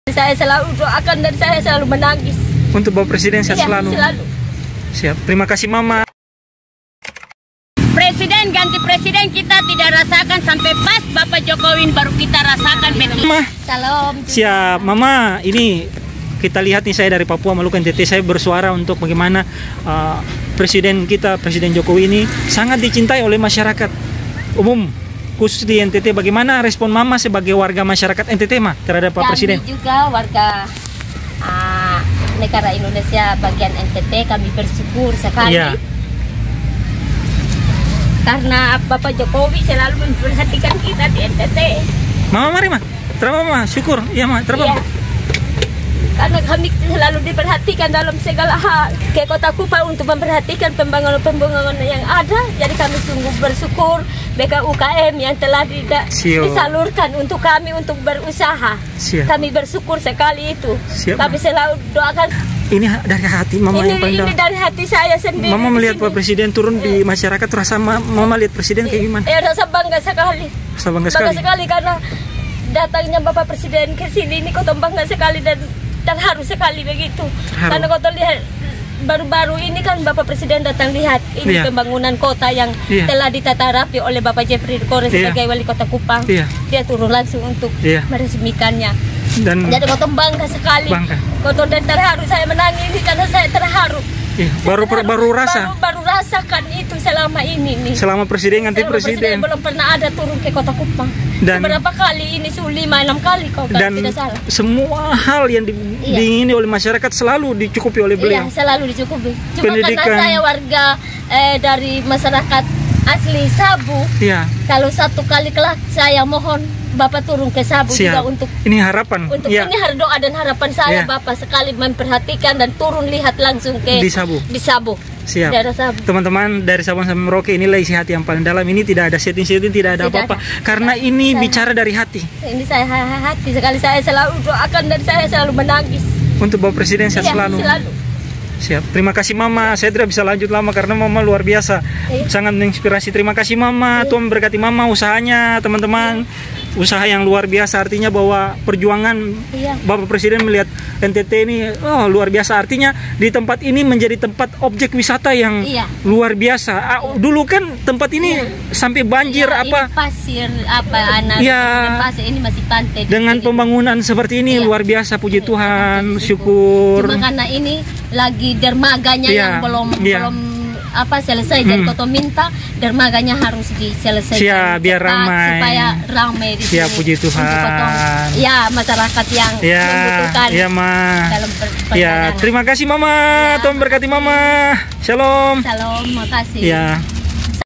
Viral ! Air Mata Mama NTT Menetes Berdoa Untuk Pak Presiden Jokowi Sehat dan Selalu Hadir di NTT
Konten terbaru di channel ini, yang berisikan Berita Informasi Politik, Sosial, Ekonomi & Hukum.